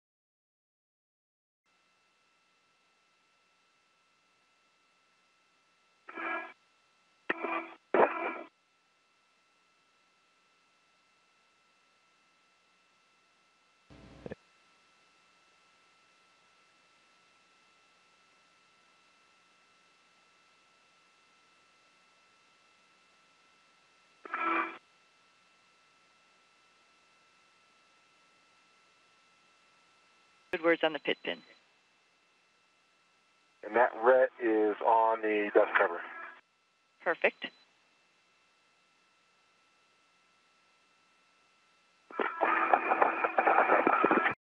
Nouvelle sortie orbitale de deux astronautes sur l'ISS
Deux astronautes des six membres d'équipage de la Station spatiale internationale (ISS) effectuent dimanche une nouvelle sortie orbitale, la troisième depuis une semaine, pour installer des équipements nécessaires à l'amarrage des futurs vaisseaux commerciaux américains qui transporteront des astronautes à partir de 2017. Durée: 00:44